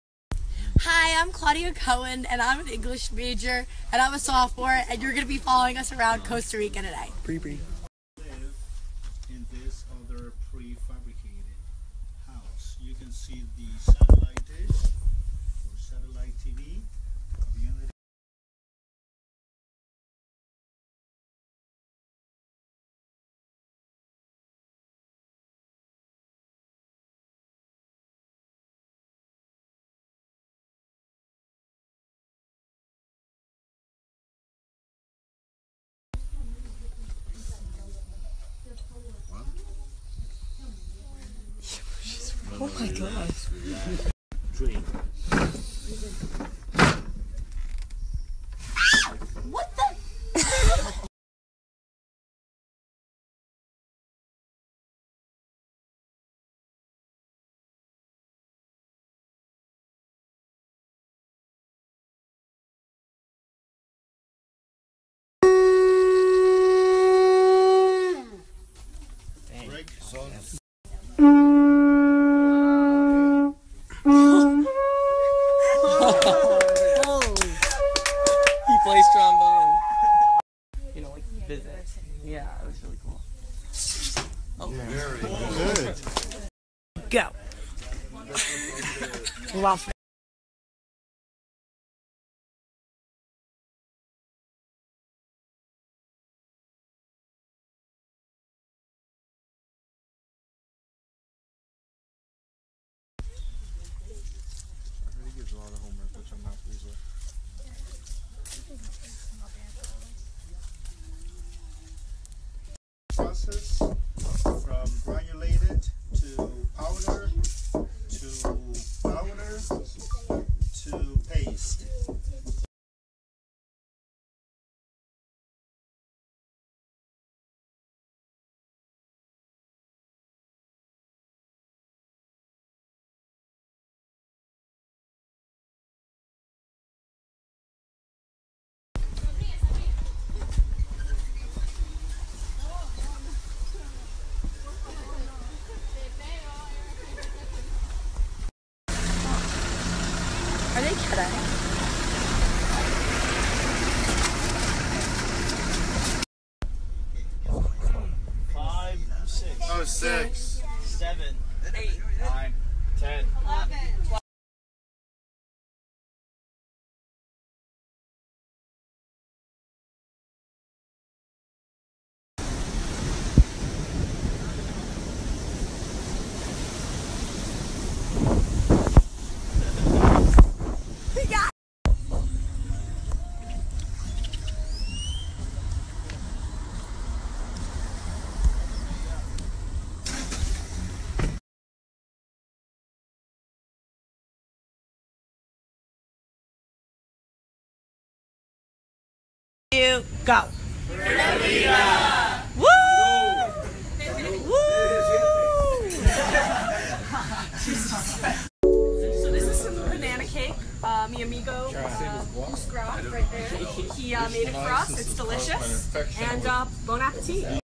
LehighU Snapchat story | Abroad in Costa Rica
Follow Lehigh students on their adventures while abroad in Costa Rica!